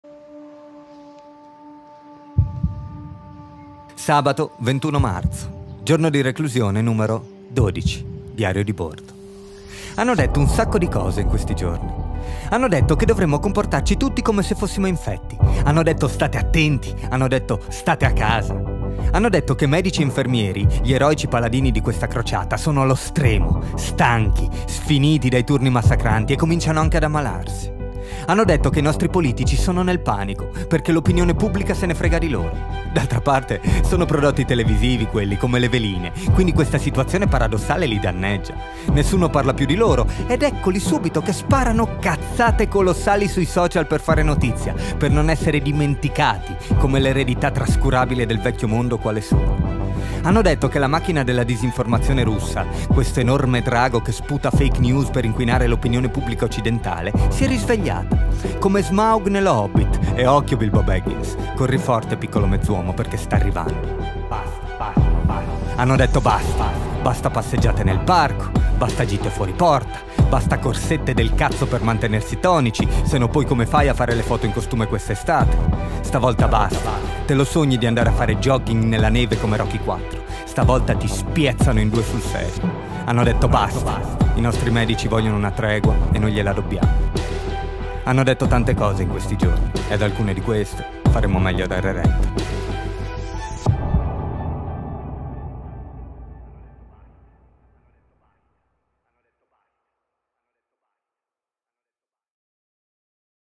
DIARIO DI BORDO - Giorno 12
Quello che dice il titolo, niente di più, niente di meno. Pensieri, riflessioni, suoni e immagini per dare a questo momento epocale che stiamo vivendo, un motivo in più per essere ricordato.

Le parole sono di Doro Gjat (IG: @dorogjat)
I suoni sono di Jamie Fields (IG: @iwantallthemcnuggets)

Hanno detto un sacco di cose in questi giorni. Hanno detto che dovremmo comportarci tutti come se fossimo infetti, hanno detto “state attenti”, hanno detto “state a casa”. Hanno detto che medici e infermieri, gli eroici paladini di questa crociata, sono allo stremo, stanchi, sfiniti dai turni massacranti, e cominciano anche ad ammalarsi. Hanno detto che i nostri politici sono nel panico perché l’opinione pubblica se ne frega di loro. D’altra parte sono prodotti televisivi quelli, come le veline, quindi questa situazione paradossale li danneggia. Nessuno parla più di loro ed eccoli subito che sparano cazzate colossali sui social per fare notizia, per non essere dimenticati come un'eredità trascurabile del vecchio mondo quale sono. Hanno detto che la macchina della disinformazione russa, questo enorme drago che sputa fake news per inquinare l’opinione pubblica occidentale, si è risvegliata come Smaug ne Lo Hobbit e occhio Bilbo Baggins, corri forte piccolo mezzuomo, perché sta arrivando. Hanno detto basta, basta passeggiate nel parco, basta gite fuori porta, basta corsette per mantenersi tonici, sennò poi come fai a fare le foto in costume quest’estate. Stavolta basta, te lo sogni di andare a fare jogging nella neve come Rocky IV. Stavolta ti spiezzano in due sul serio. Hanno detto basta, i nostri medici vogliono una tregua e noi glie la dobbiamo. Hanno detto tante cose in questi giorni e ad alcune di queste faremmo meglio a dare retta.